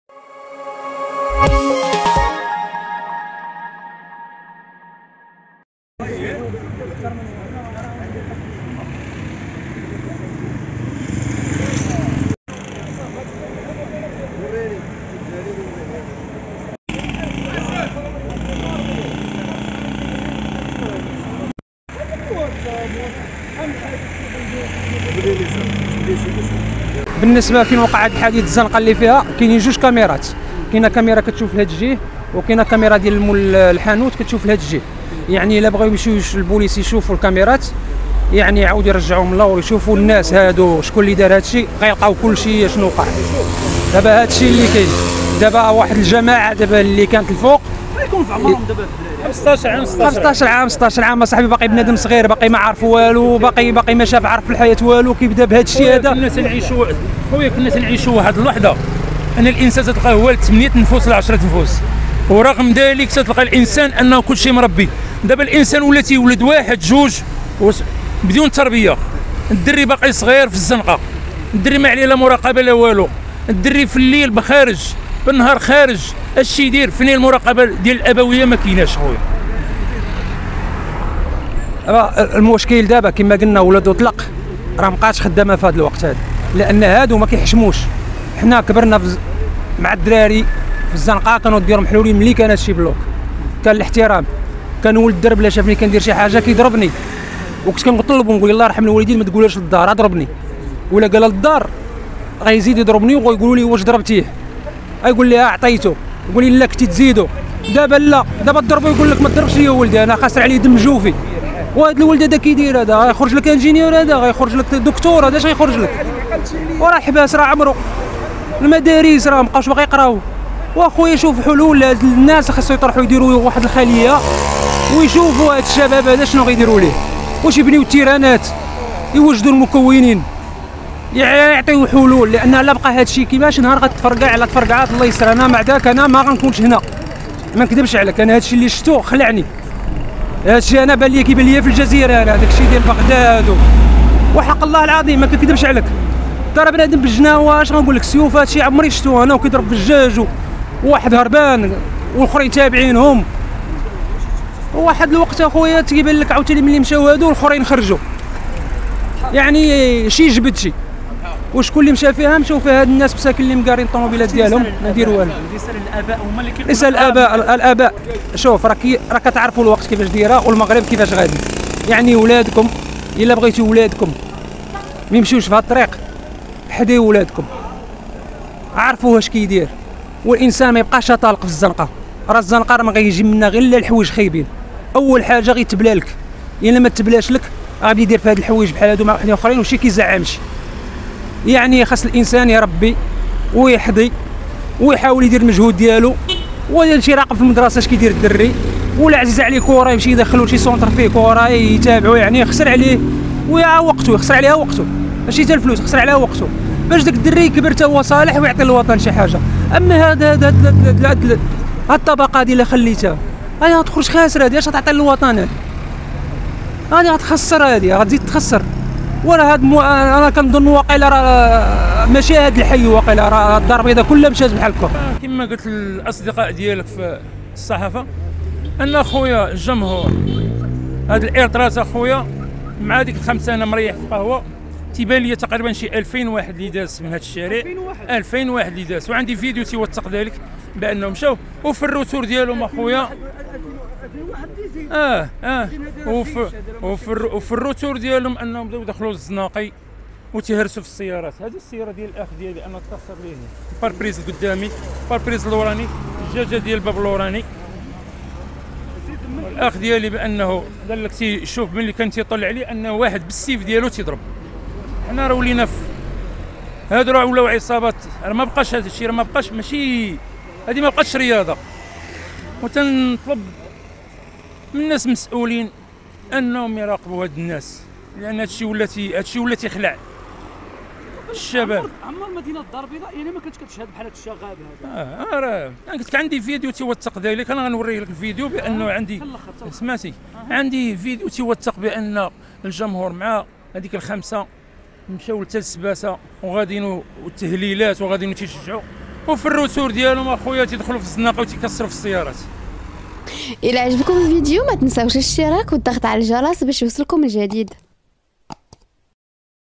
"تخريب سيارات والمواطنين يحتجون ويوجهون رسالة قوية إلى عناصر الأمن "راه صحاب الإلترات تعداو علينا
تخريب سيارات و المواطنين يحتجون و يوجهون رسالة قوية إلى الأباء راه صحاب الإلترات تعداو علينا